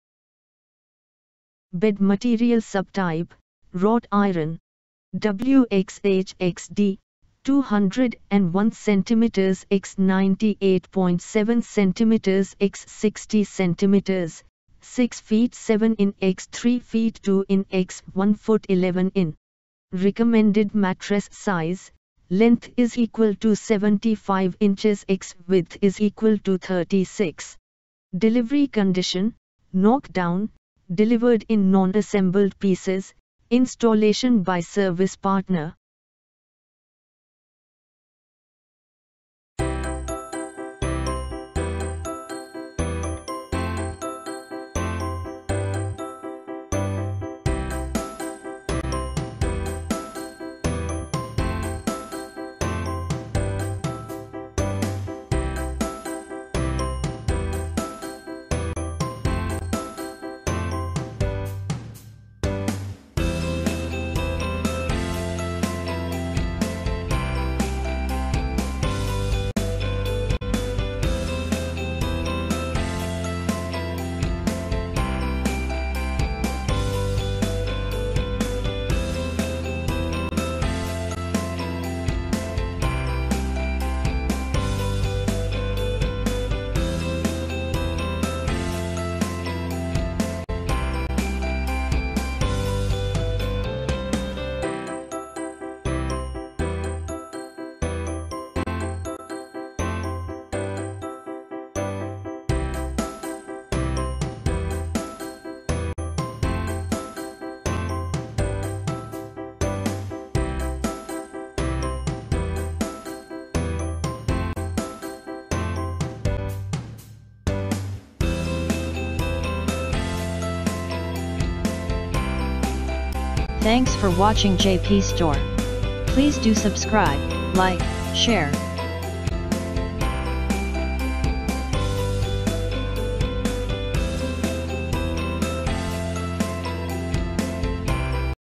FurnitureKraft Sydney Metal Single Bed Finish Color Black
Bed Material Subtype: Wrought Iron
W x H x D: 201 cm x 98.7 cm x 60 cm (6 ft 7 in x 3 ft 2 in x 1 ft 11 in)
Recommended Mattress Size: Length =75 inches x Width = 36
Delivery Condition: Knock Down - Delivered in non-assembled pieces, installation by service partner